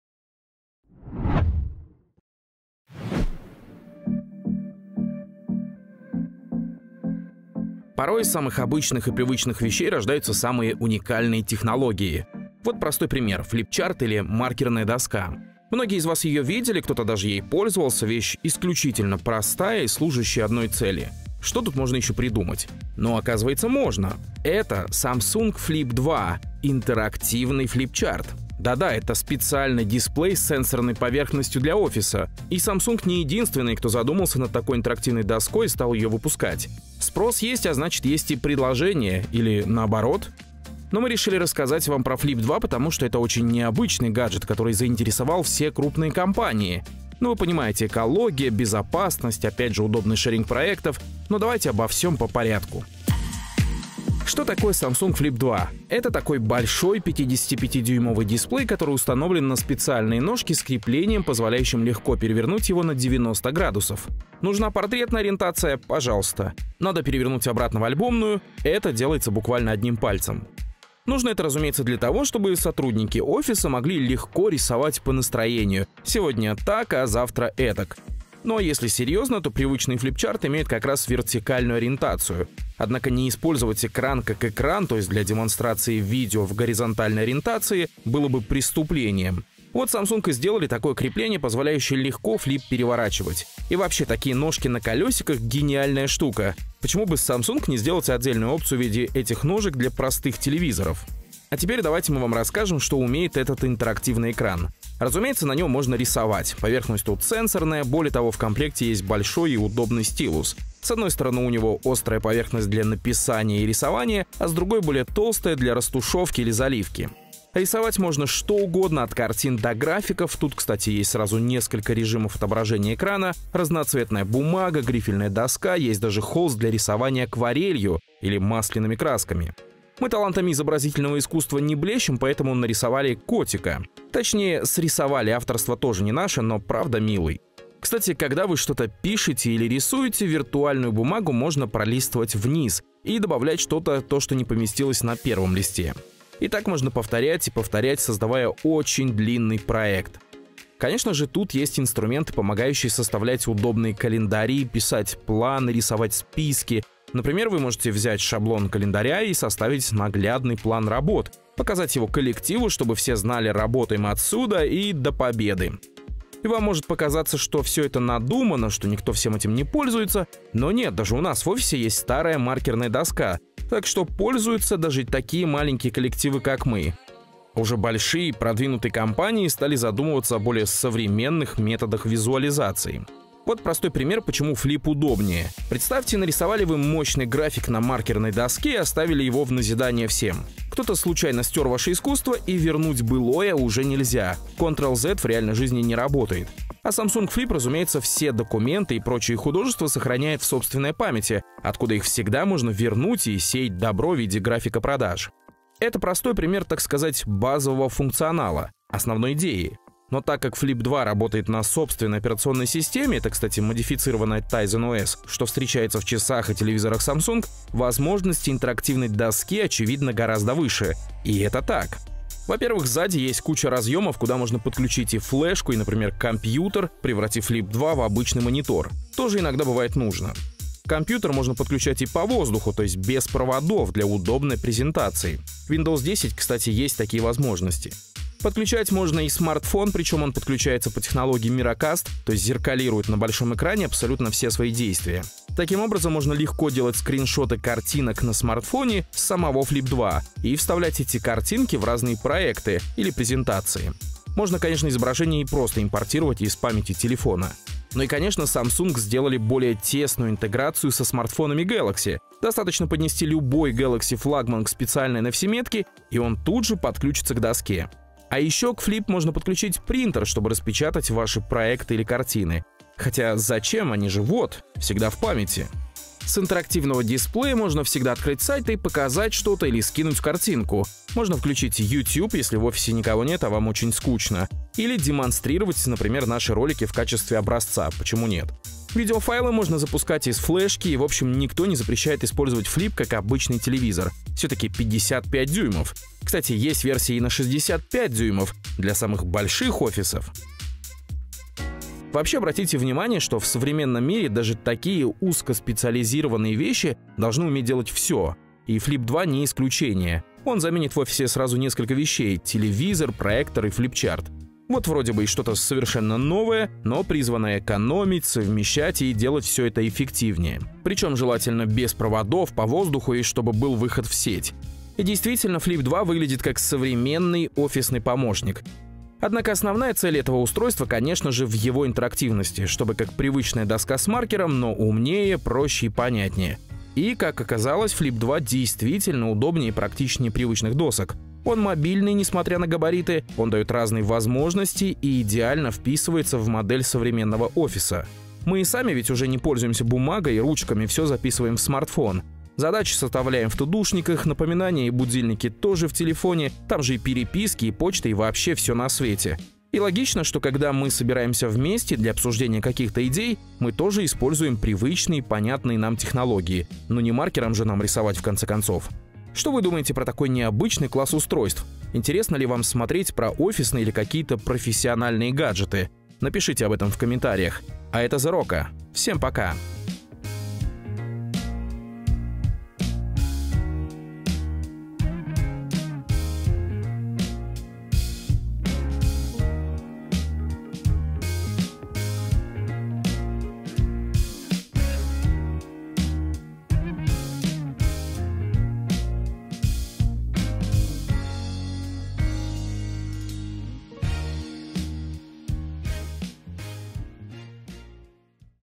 Интерактивный флипчарт Samsung FLIP 2 - такое вы еще не видели!
Такой гаджет вы еще не видели - офисный помощник Samsung Flip 2. Это настоящий интерактивный флипчарт!

Партнерский материал, создан при поддержке Samsung.

Samsung Flip 2 - это цифровая маркерная доска или интерактивный флипчарт. И это прекрасный помощник, как для офиса, так и для магазинов или кафе.